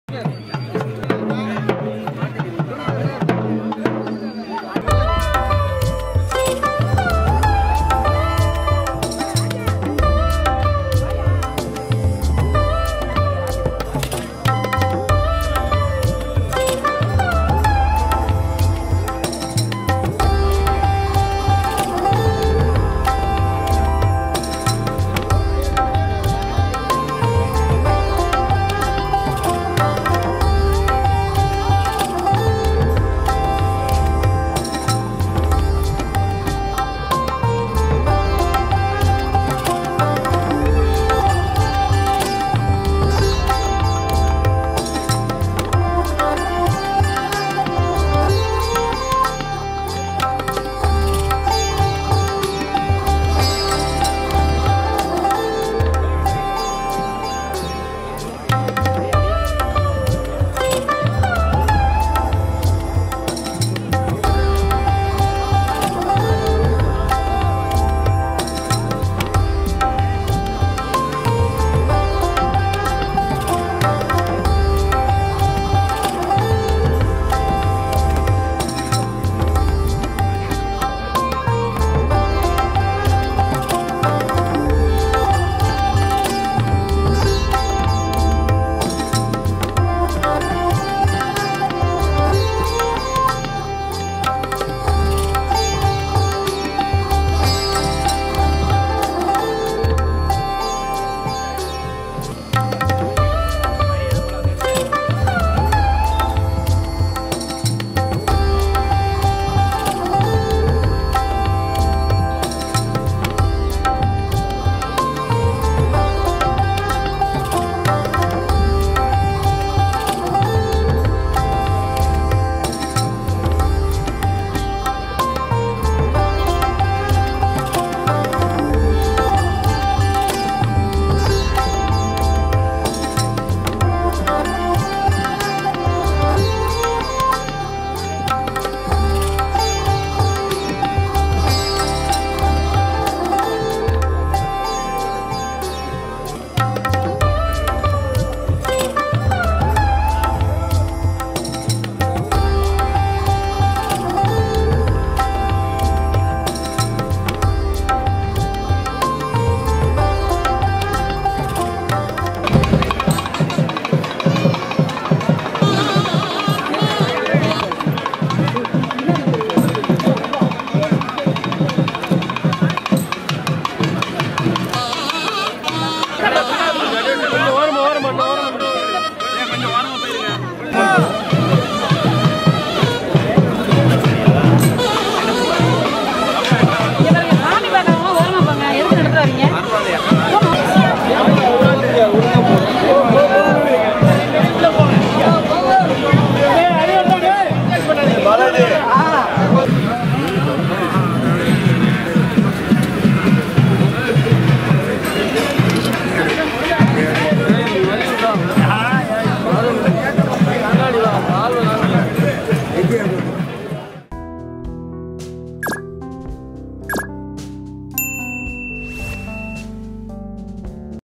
ஸ்ரீரங்கம் நம்பெருமாள் தங்க கருட சேவை
ஸ்ரீரங்கம் கோயில் சித்திரை திருவிழாவையொட்டி நம்பெருமாள் தங்க கருட சேவை - பல்லாயிரக்கணக்கான பக்தர்கள் தரிசனம்.